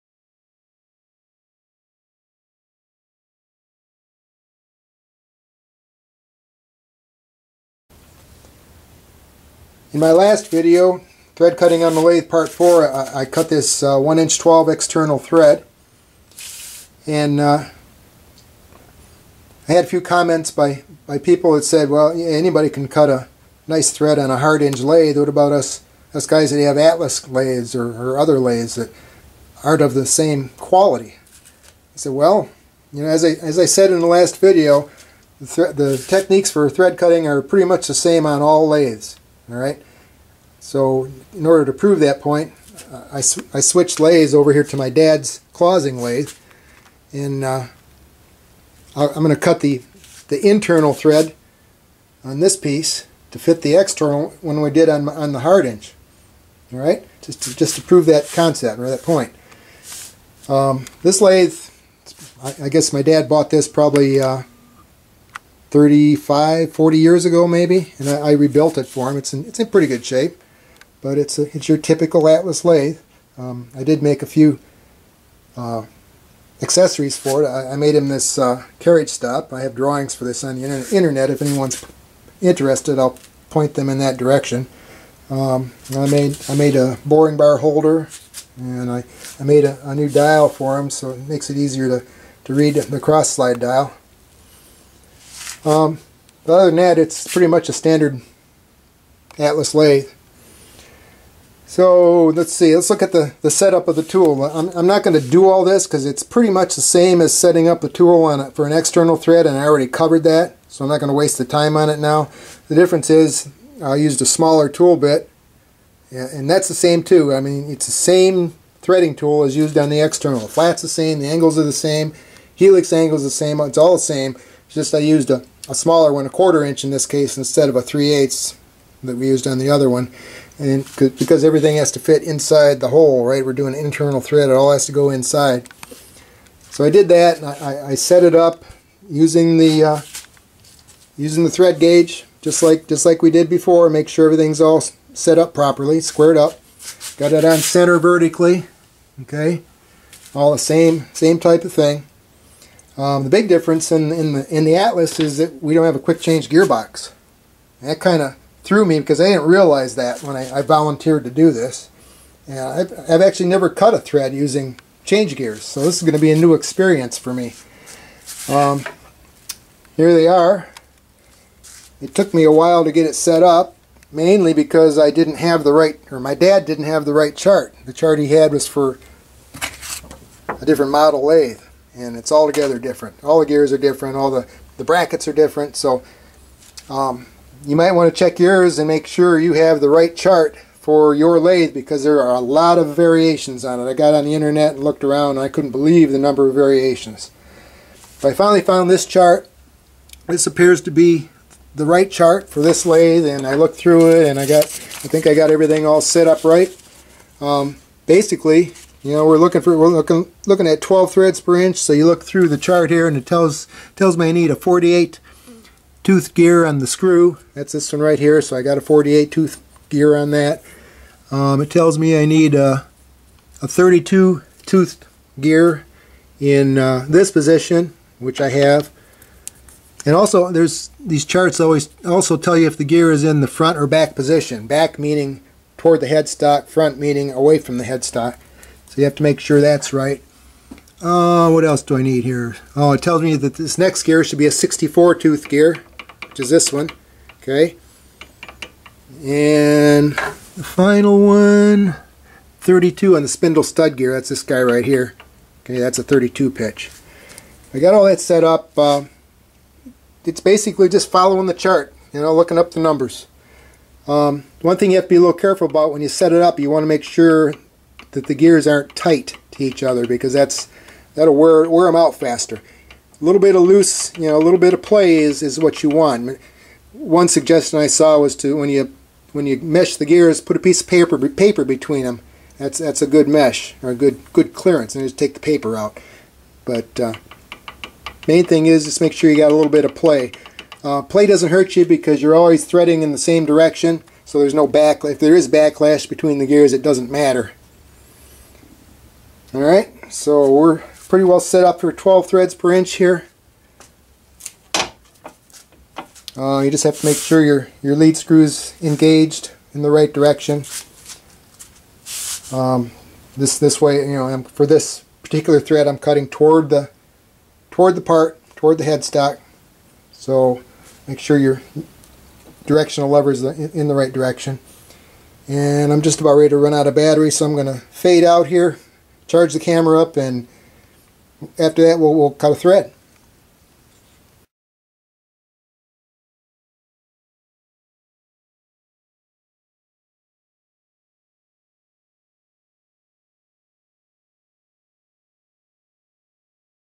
Cutting an Internal Thread on the Atlas Lathe, Part One - Setting up the Change Gears
This first of a three part series on cutting an internal thread on the Atlas lathe, covers setting up the change gears to cut a twelve pitch thread. I discuss how to read the chart and to properly set the backlash in the gears.